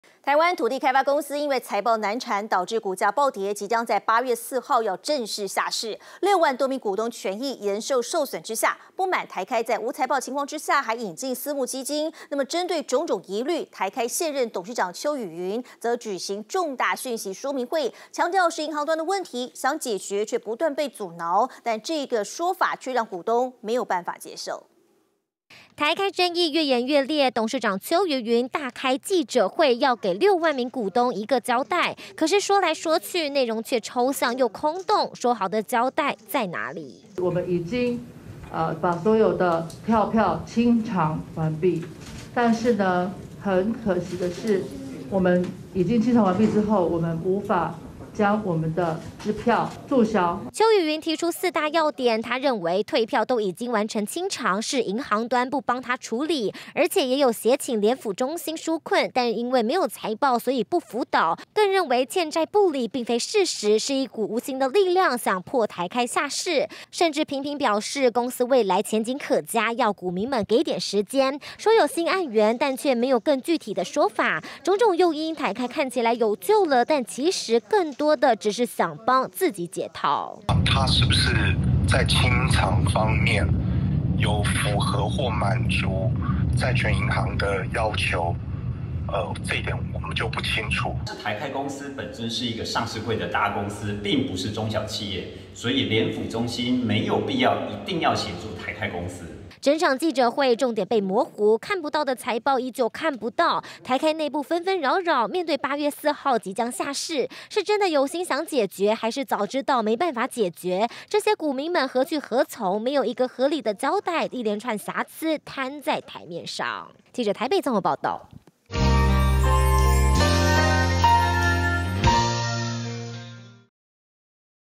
台開還有救？ 董座邱于芸急跳腳 喊冤：是銀行端不給註銷
台灣土地開發公司，因財報難產導致股價暴跌，即將在8月4日下市，6萬多名股東權益嚴重受損，不滿台開在無財報的情況下，引進私募資金，種種疑慮台開現任董事長邱于芸，舉行重大訊息說明會。只不過內容說來說去，都認為是銀行端的問題，公司努力想解決卻一直被阻饒。